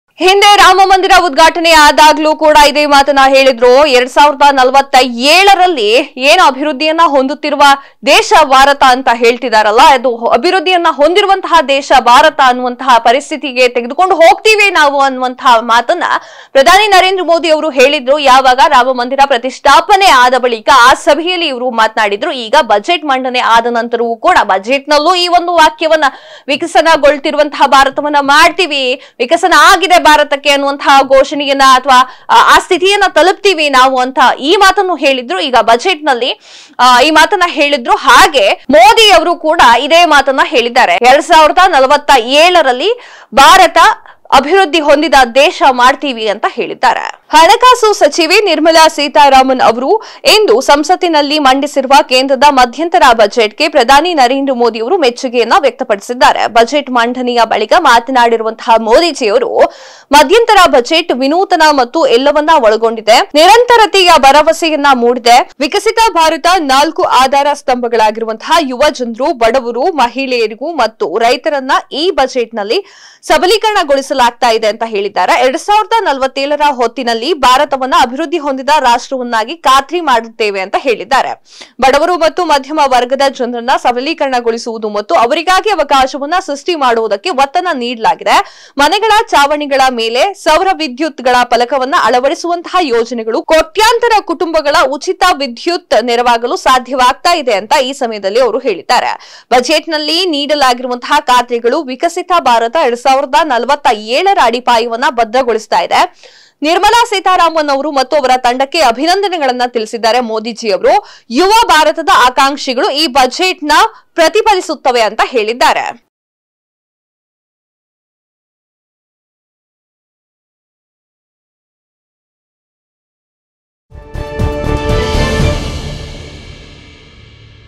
2047ರ ಹೊತ್ತಿಗೆ ವಿಕಸಿತ ಭಾರತ' ಖಾತ್ರಿಪಡಿಸಿದ ಬಜೆಟ್‌: PM | Karnataka TV
2047ರ ಹೊತ್ತಿಗೆ ವಿಕಸಿತ ಭಾರತ' ಖಾತ್ರಿಪಡಿಸಿದ ಬಜೆಟ್‌: PM  | Karnataka TV